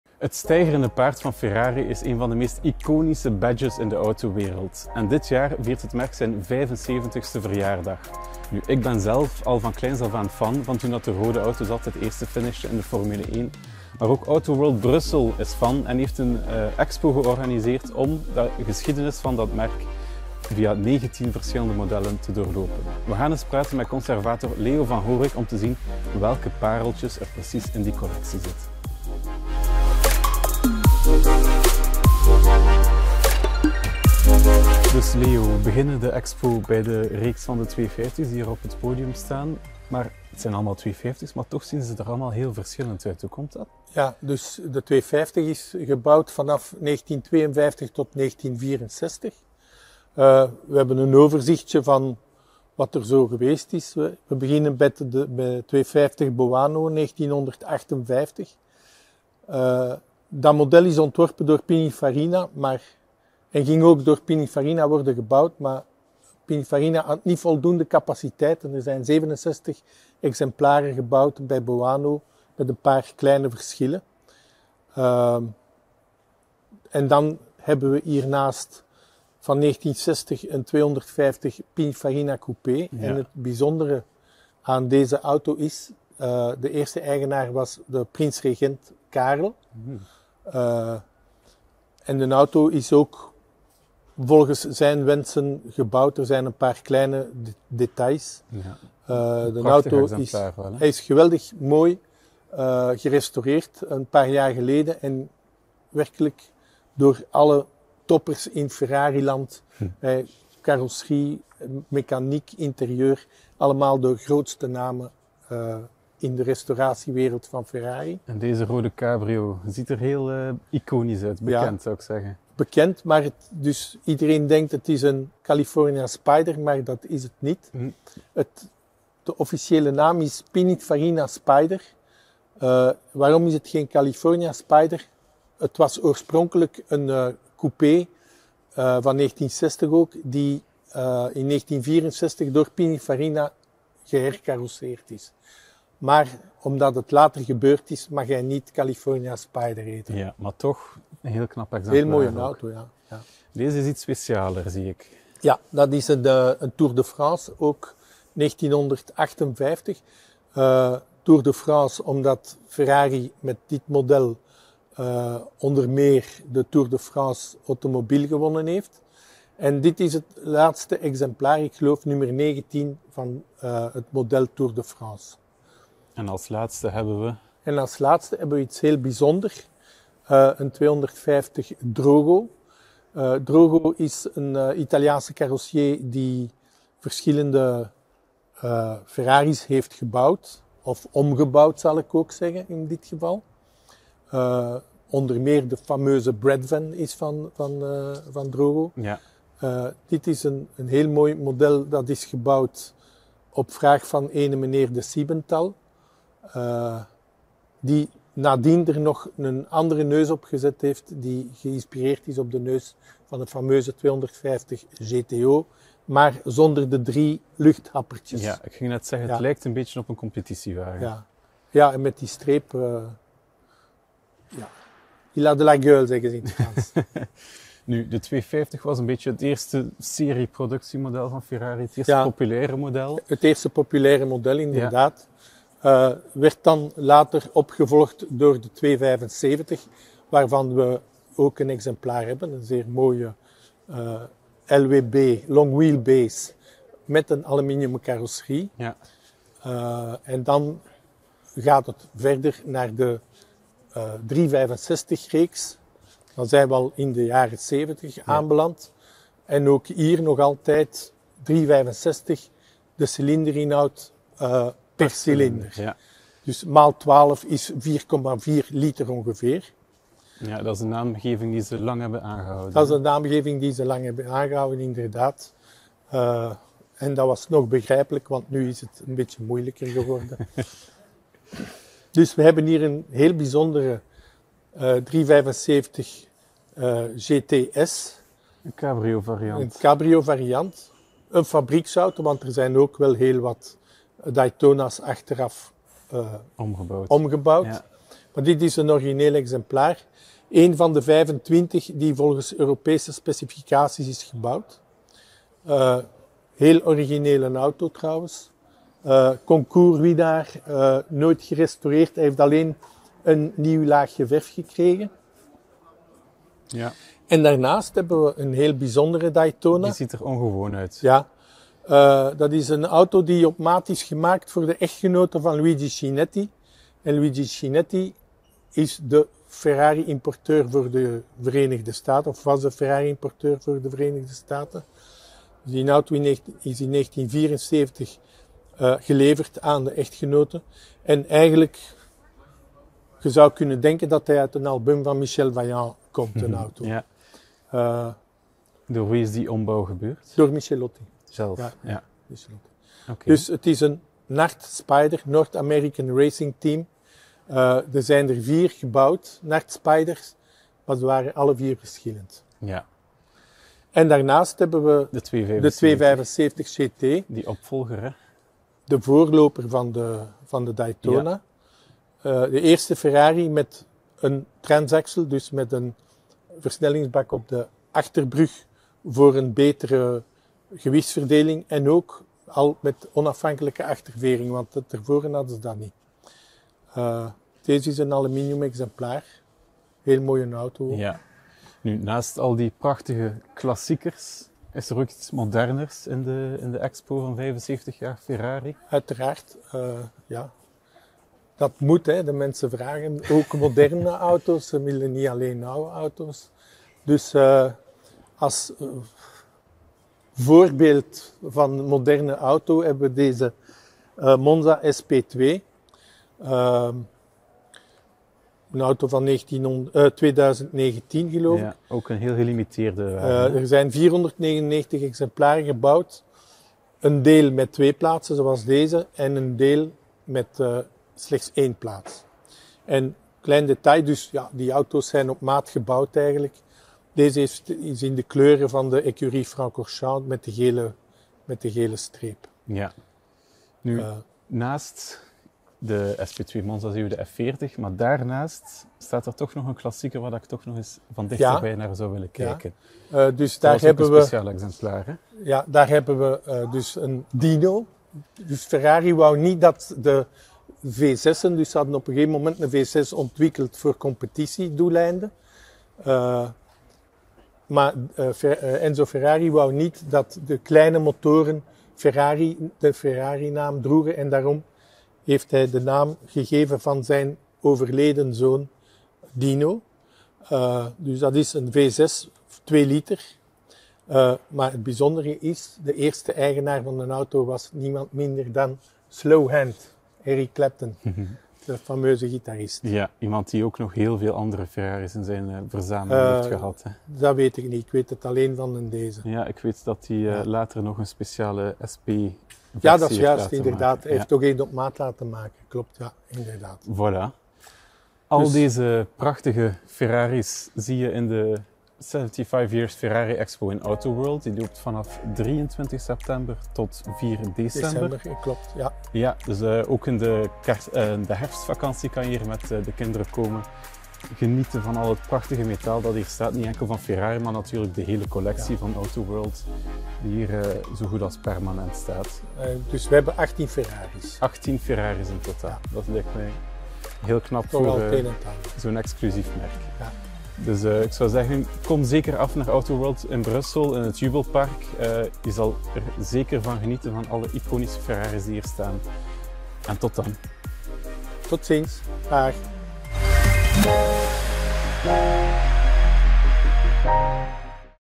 Expo 75 anni di Ferrari in AutoWorld Brussels
Ferrari is 75 jaar jong en dat verdient een feestje. Zoals in AutoWorld Brussels, waar de Expo '75 anni di Ferrari' georganiseerd wordt van vrijdag 23 september tot zondag 4 december 2022. AutoGids ging al eens kijken en nam een videocamera mee...

Autoworld stelt gedurende meer dan twee maanden een twintigtal uitzonderlijke Ferrari's tentoon. De « crème de la crème » van het merk ! Elk van hen heeft een bijzonder verhaal. Ze zijn historisch bijzonder, werden slechts in een heel klein aantal exemplaren geproduceerd, dienden ooit een bekende autocoureur of waren eigendom van een beroemdheid ...  Zo kunnen we onder andere volgende Ferrari’s bewonderen : de Dino 206 (voorheen Clapton), de unieke Daytona Michelotti, de 195 Inter Ghia, de 500 Superfast (van Peter Sellers), de 250 GT Spider LWB, de 308GTS en nog veel meer...